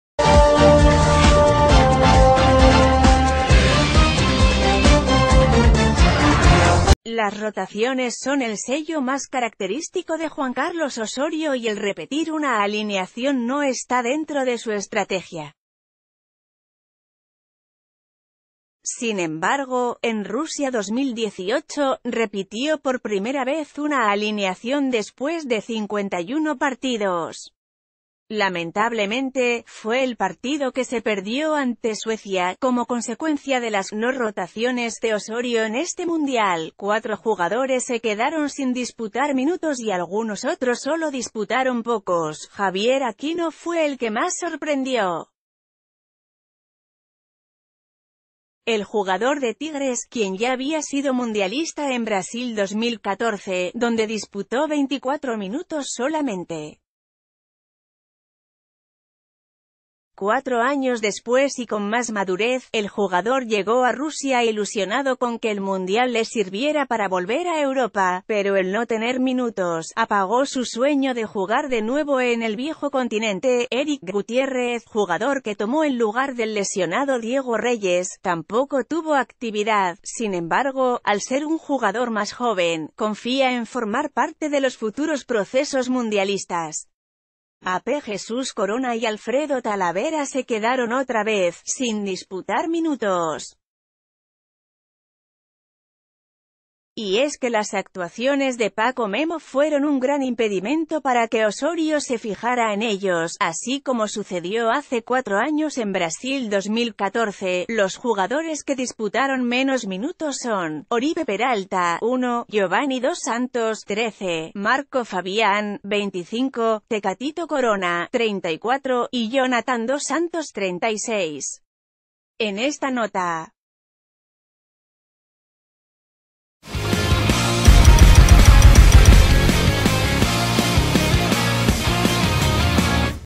Los 4 mexicanos que no disputaron ni un minuto en Rusia
Las rotaciones son el sello más característico de Juan Carlos Osorio y el repetir una alineación no está dentro de su estrategia. Sin embargo, en Rusia 2018, repitió por primera vez una alineación después de 51 partidos. Lamentablemente, fue el partido que se perdió ante Suecia. Como consecuencia de...